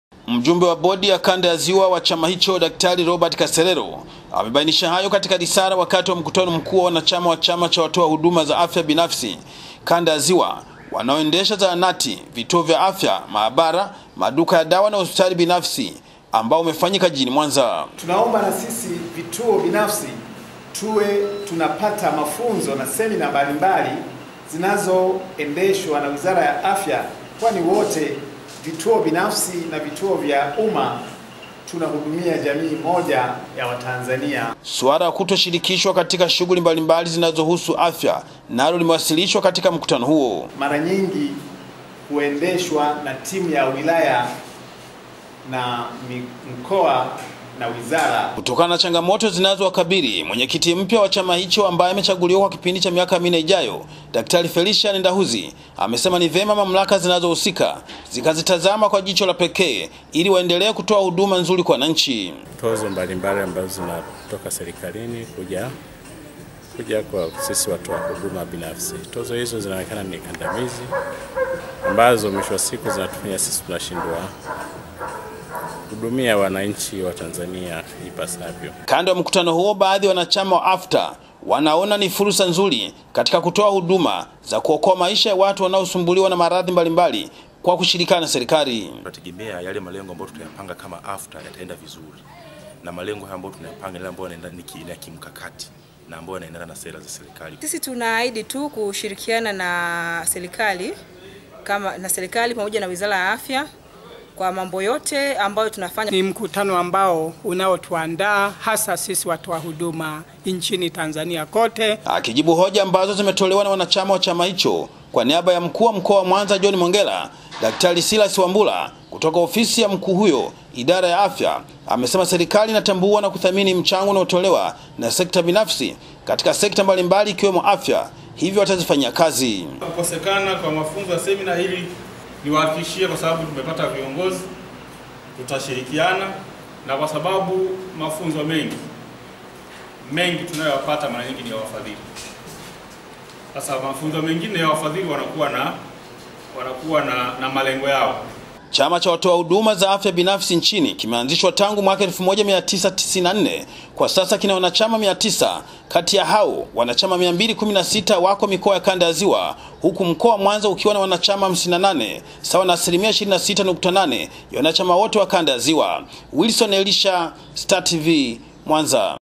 WATOA HUDUMA BINAFSI ZA AFYA WATUMA OMBI KWA SERIKALI.
Chama cha watoa huduma binafsi za Afya nchini APHFTA kanda ya ziwa  kimeiomba serikali kutoa mafunzo ya mara kwa mara kwa wanachama wao ili kuwajengea wa kufanya kazi tija.